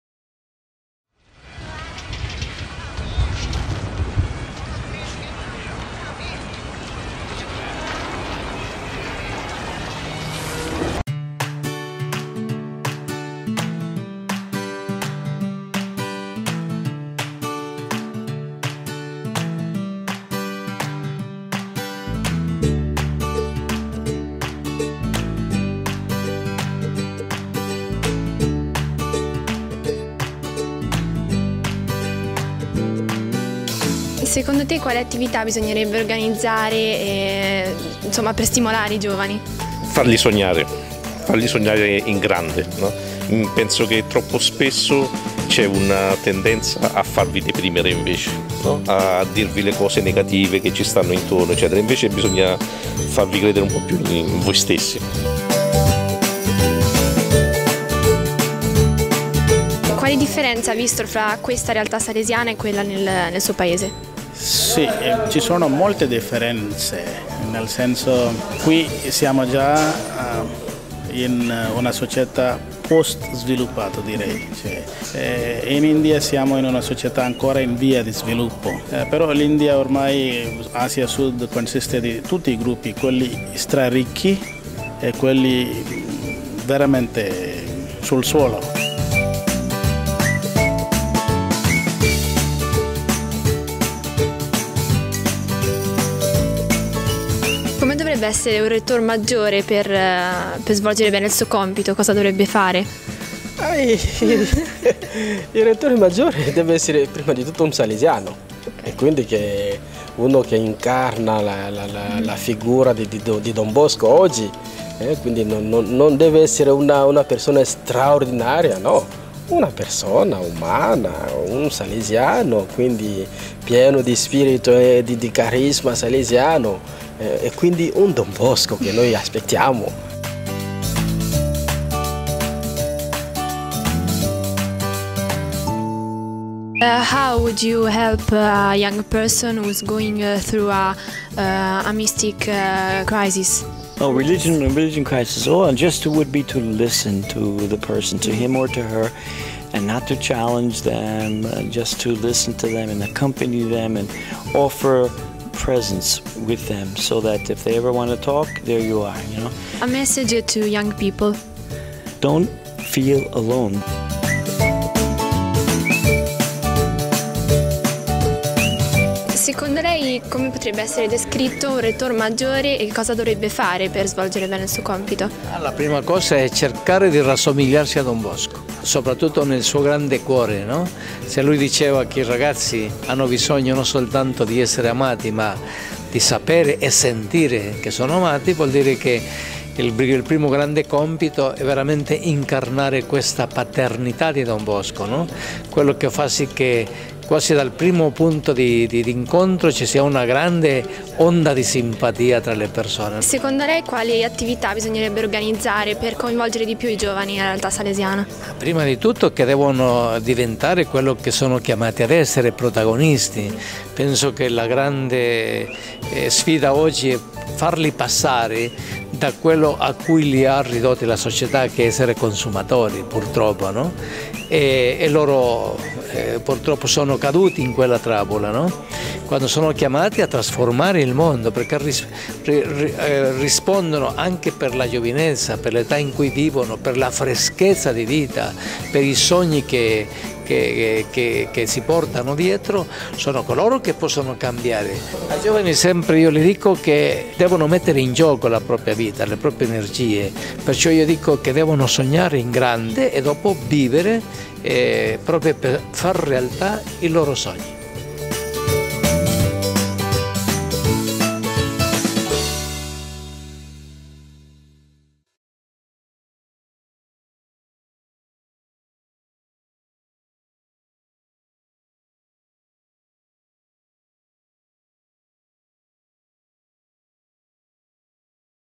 Con i tuoi Occhi (Capitolo Generale 27) - Don Chávez
Video interviste realizzate ai partecipanti del Capitolo Generale 27 dai giovani del Cinecircolo Giovanile Salesiano "Dorico" di Ancona. Intervento finale di Don Pascual Chávez, Rettor Maggiore (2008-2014)